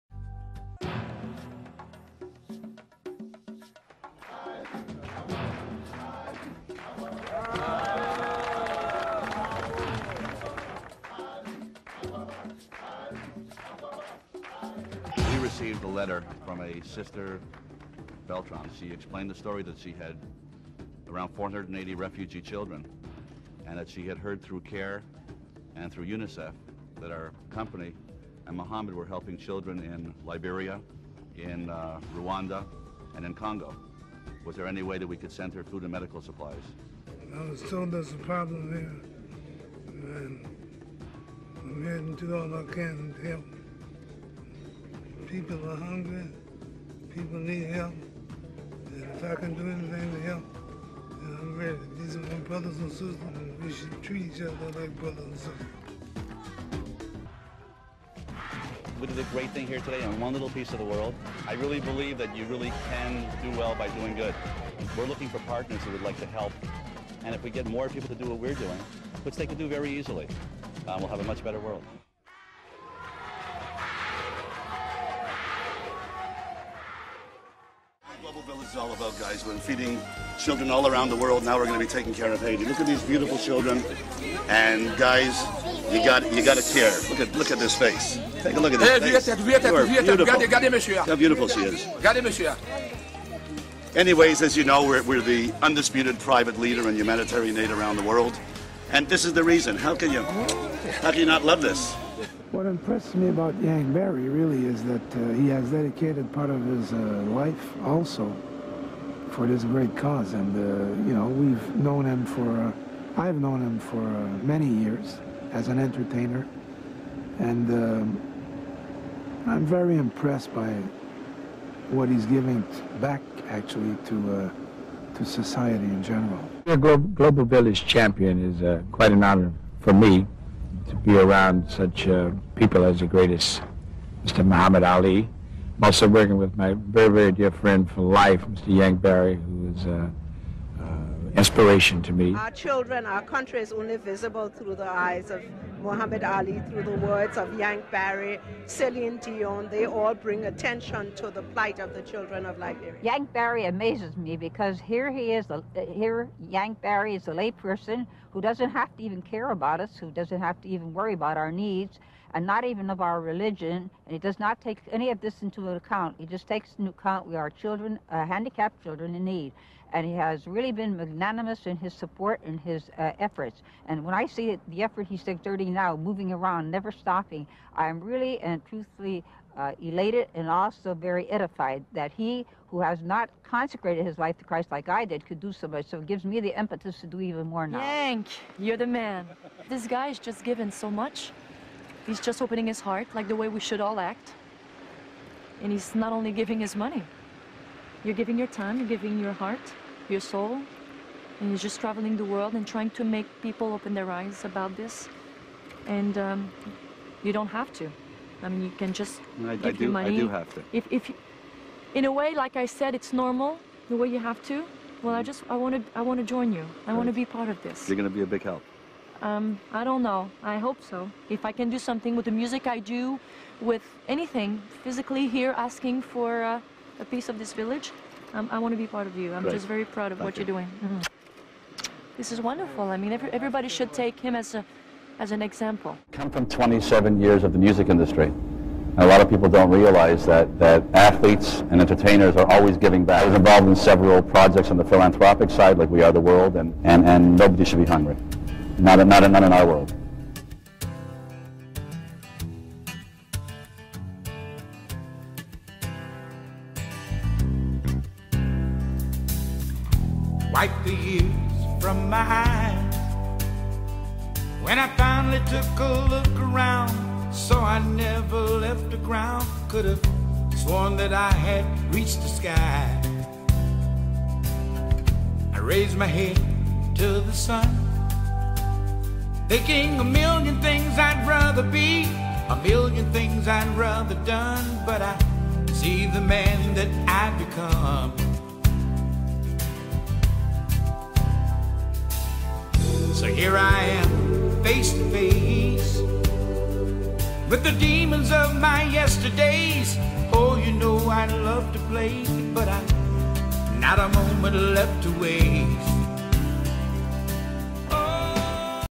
Yank Barry - short bio
Short biography featuring Yank and Global Village Champions Muhammad Ali and Gary U.S. Bonds delivering food and hope to hungry children around the World.